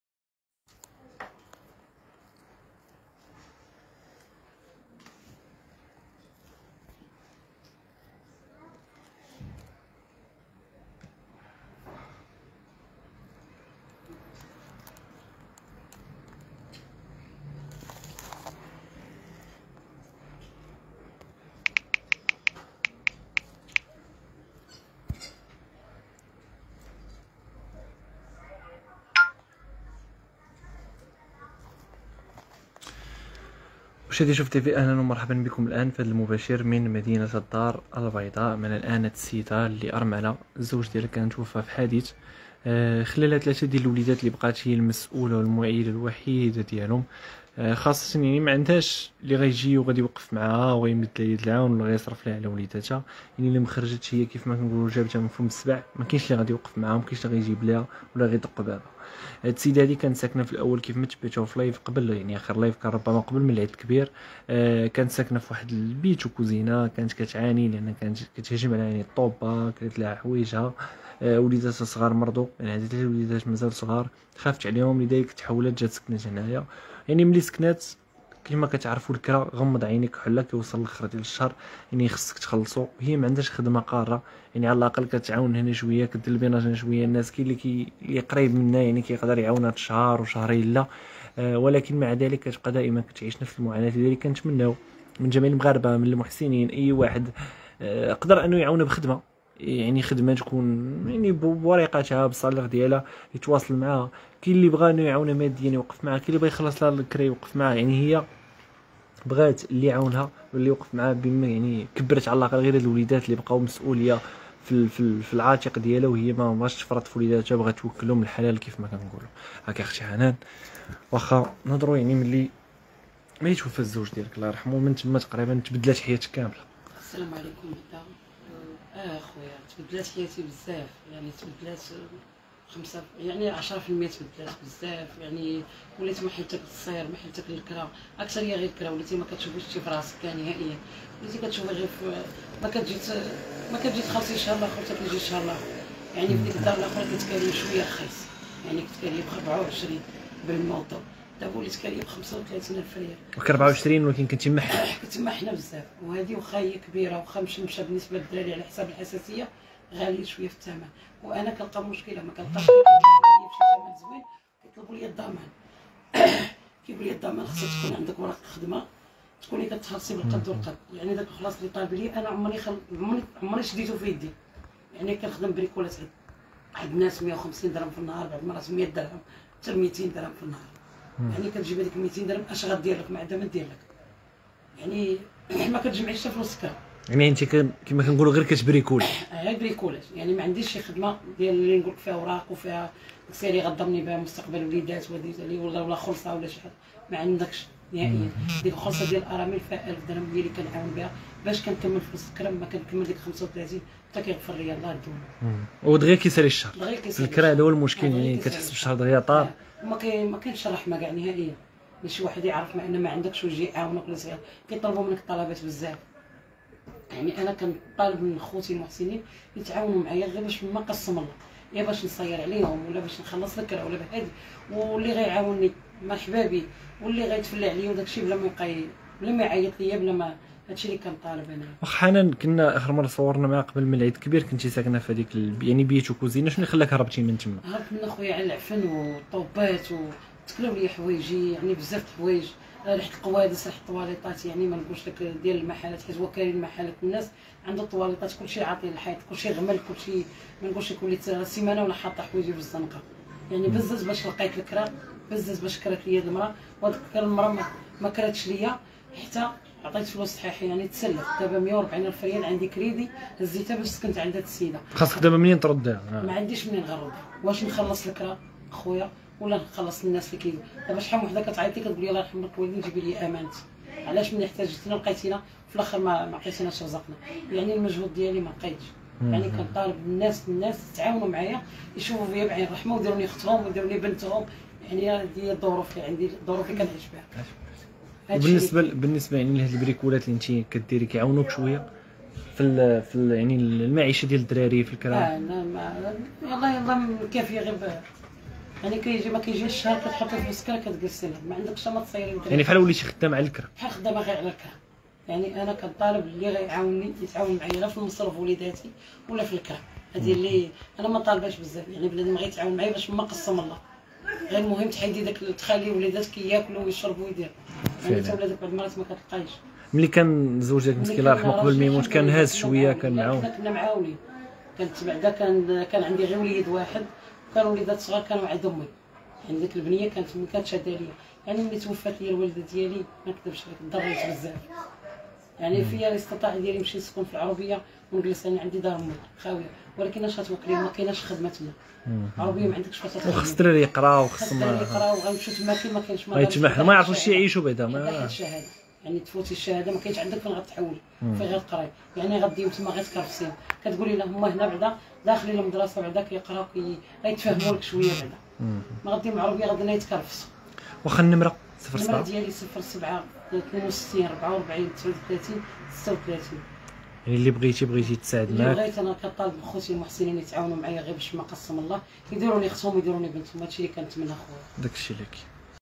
أرملة عندها 3 الوليدات توفى زوجها فكسيدة خايبة هربات من بيت فيه الطوبة ووحلات ففلوس الكراء
مباشرة من الدار البيضاء:أرملة عندها 3 الوليدات توفى زوجها فكسيدة خايبة هربات من بيت فيه الطوبة ووحلات ففلوس الكراء وحالتها ووليداتها صعيبة..شكون يعاونها؟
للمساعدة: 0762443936